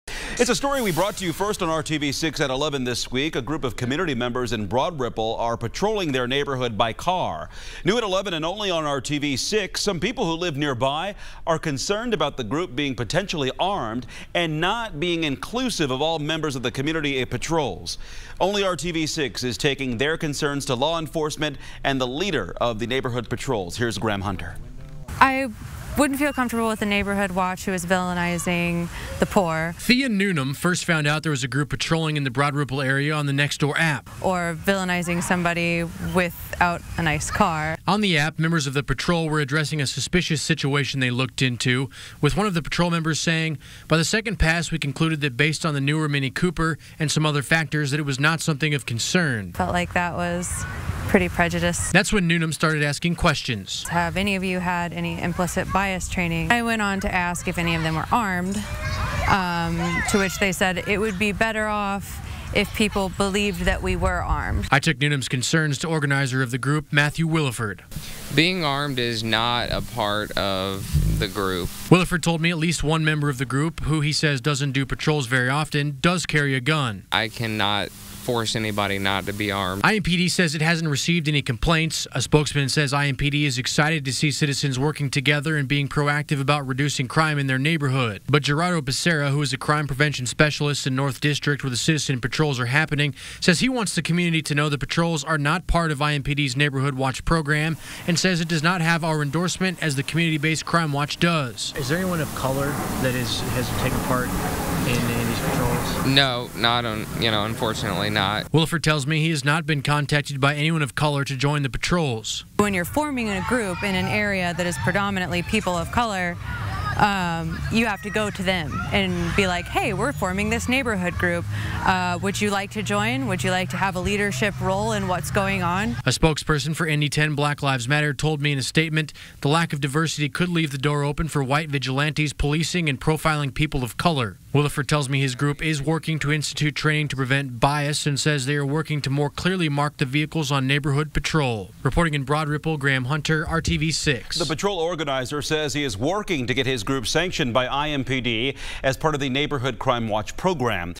People in Broad Ripple are concerned about citizen patrols
People who live in the community are concerned about their neighborhood crime watch members not having proper training and being armed.